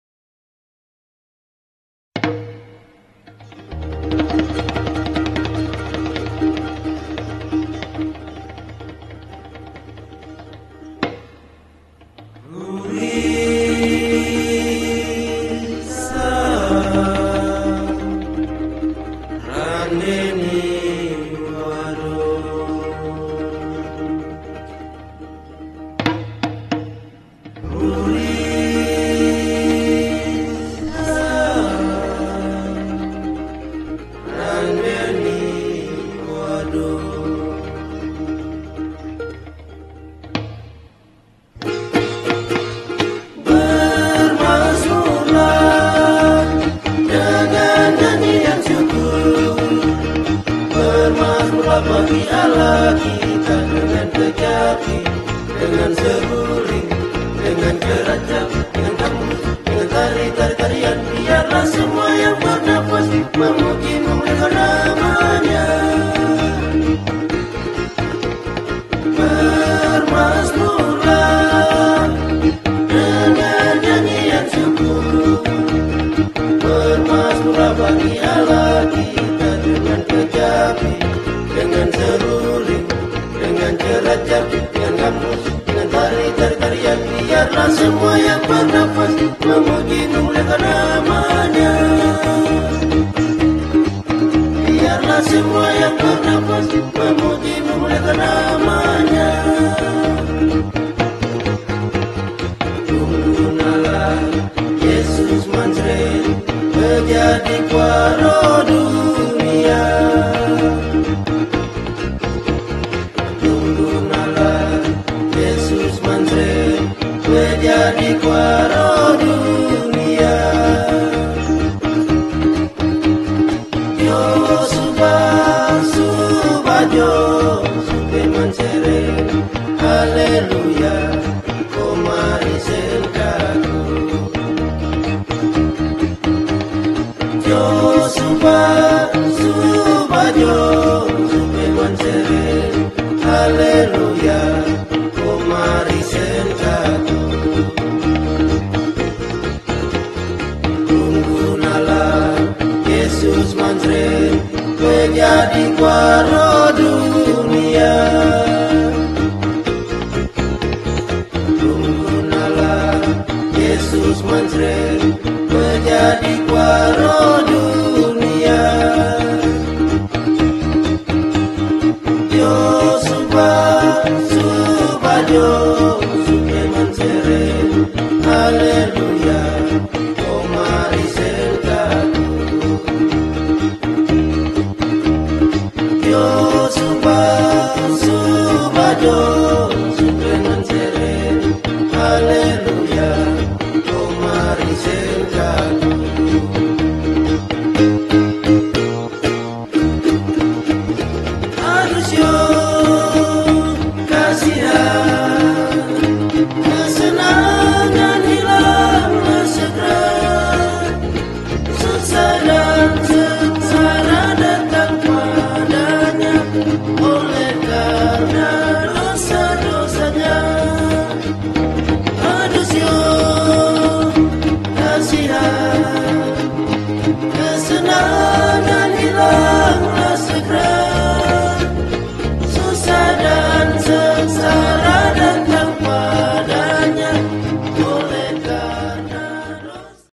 SD YPK MARTIN LUTHER SENTANI
Siswa kelas 3 Recreasi di kolam waterpalk